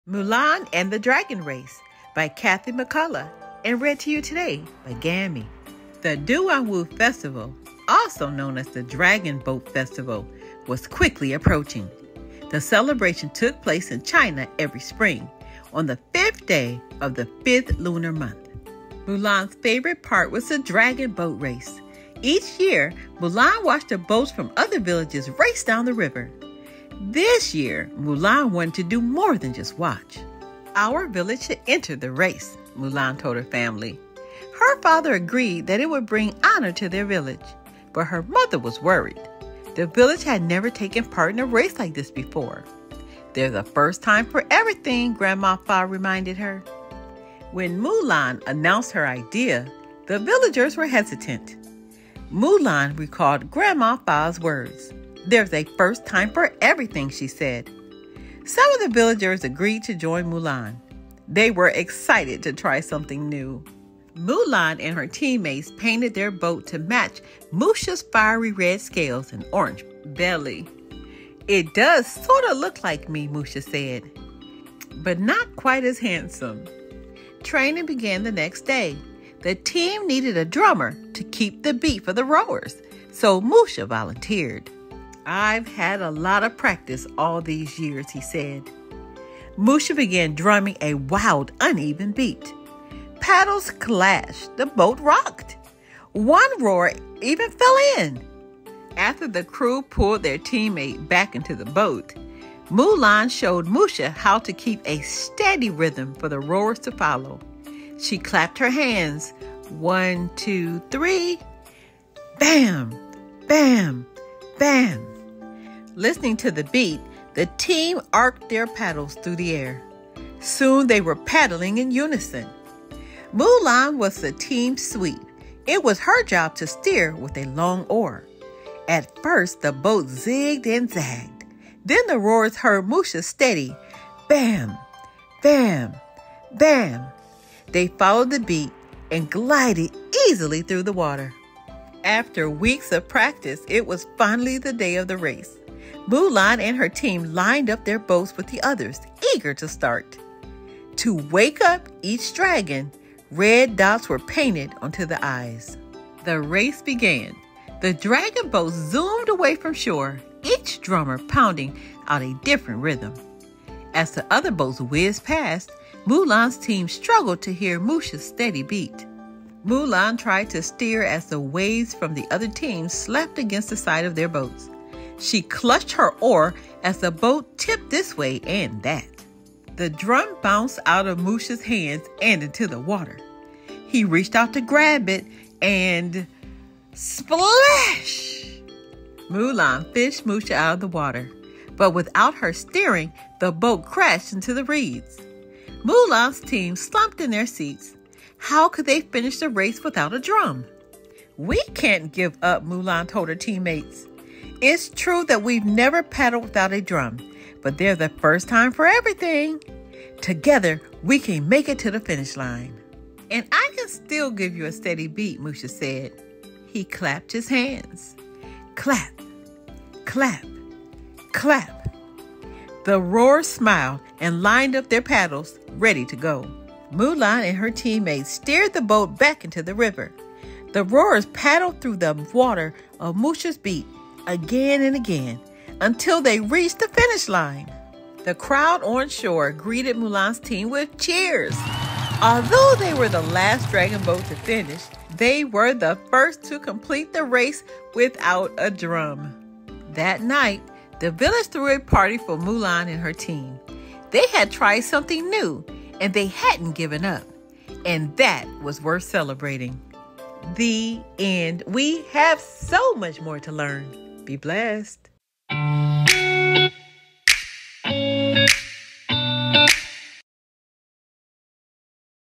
📚Mulan and the Dragon Race | Children’s Stories Read Aloud
By Kathy McCallough
Stories read aloud that teach, inspire, and instill everything good.